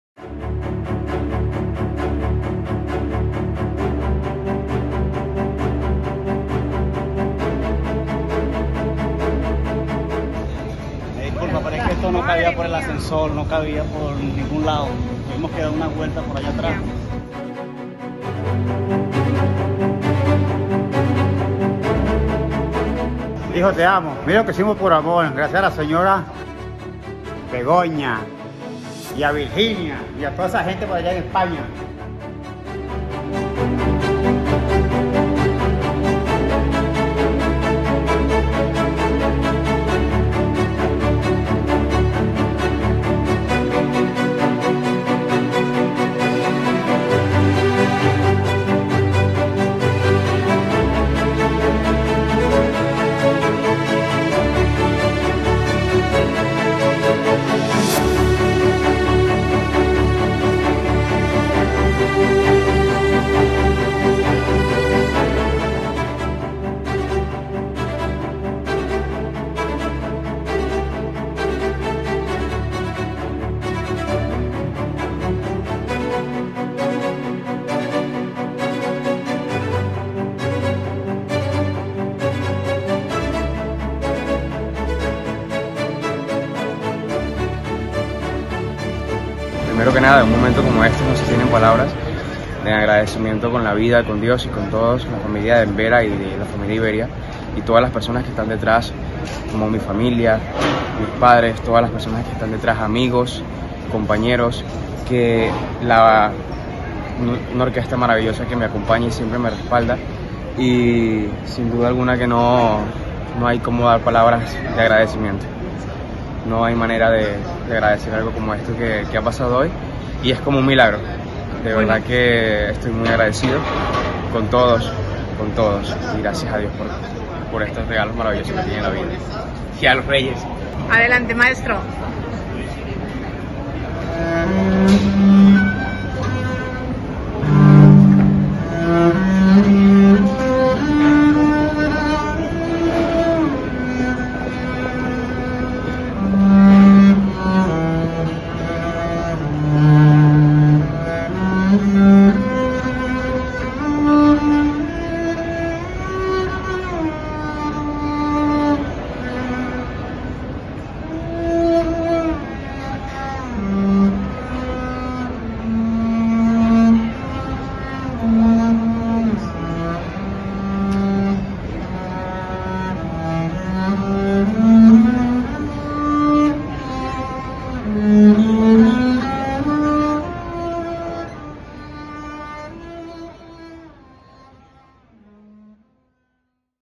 Alas comprometidas de Envera cumple el sueño de un músico venezolano en España
A primera hora del pasado 6 de enero, mientras muchas familias se despertaban con la ilusión que provoca la llegada de uno de los días más mágicos del año, una melodía recorrió la terminal 4 del Aeropuerto Adolfo Suárez Madrid – Barajas. Las notas del contrabajo de Jhorjan Bolívar anunciaban que se acababa de obrar un milagro, un sueño hecho realidad el mismo Día de Reyes.

Pero esta historia empezó hace diez meses, cuando el músico Jhorjan Bolívar salió de Venezuela a España. Una vez en Madrid, se incorporó a la Orquesta Carlos Cruz - Díez, compuesta por músicos profesionales venezolanos que en España trabajan en lo que pueden, y que lleva el nombre del artista que inundó de color la terminal del aeropuerto internacional Simón Bolívar de Caracas por donde salen tantos viajeros con la esperanza de volver algún día.